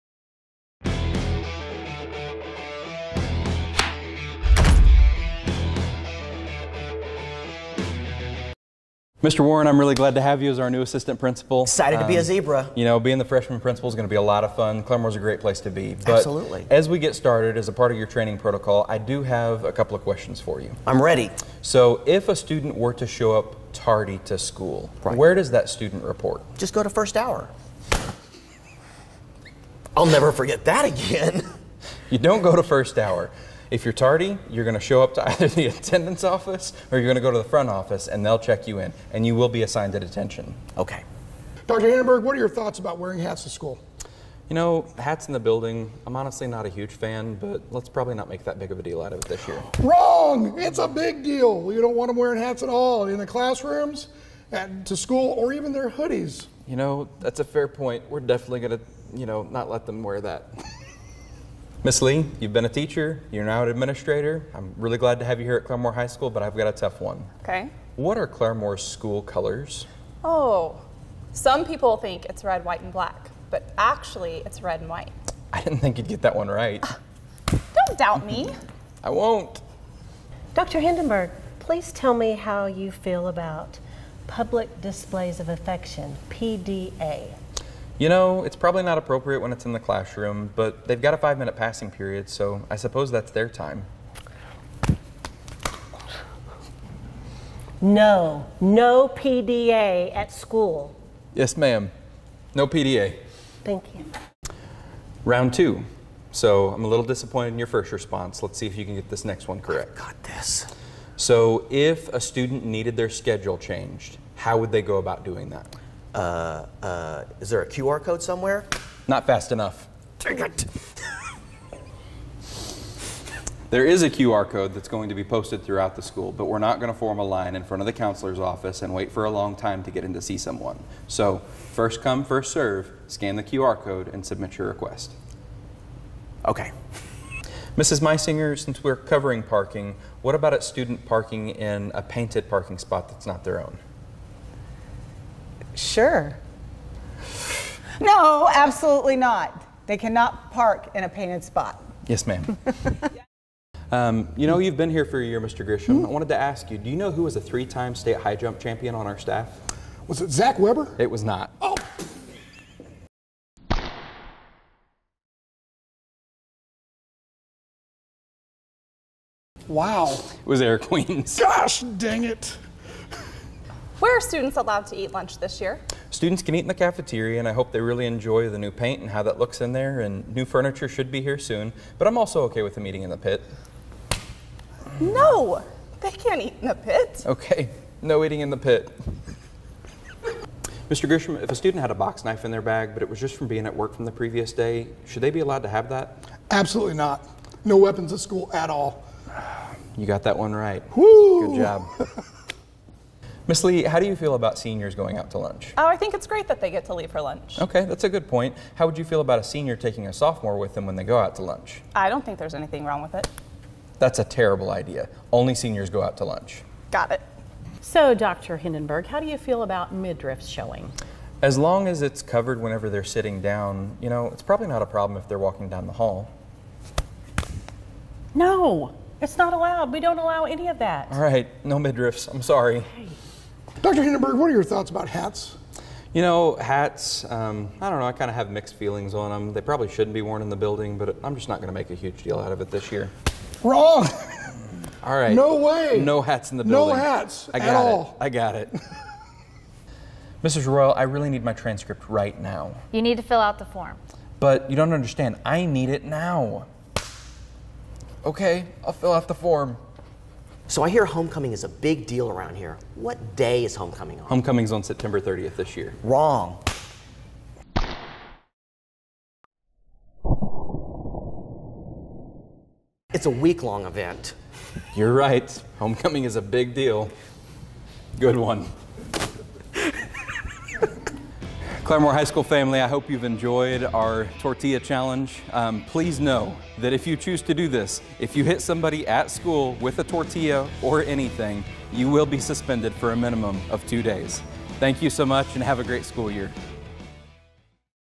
2022-2023 CHS Rules Video
Welcome back, Zebras! We are so glad to have you with us again at Claremore High School. Here Principal Hindenburg, and Assistant Principals Lee, Warren, and Grisham along with other CHS staff help us remember the rules for school.